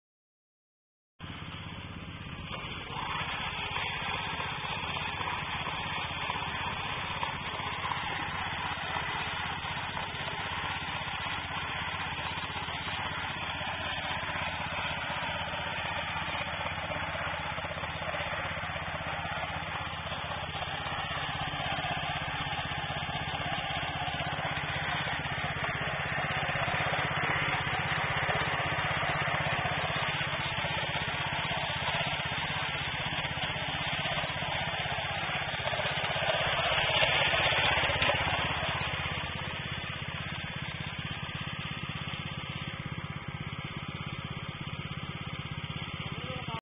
Mini hay baler wrapper
Wrap the bales made by Runshine RXYK0850 and RXYK0870 by plastic films. The packed green material can be stored for long term.
Specifications:
1.Runshine factory manufacture hay equipment, including hay wrapper RXHW-0810.
2.Model RXHW-0810 is assembled with 6HP gasoline engine.
3.Wrapping the size of bale:50x70cm and 60x70cm.
4.Manufacturer.
5.High working efficiency.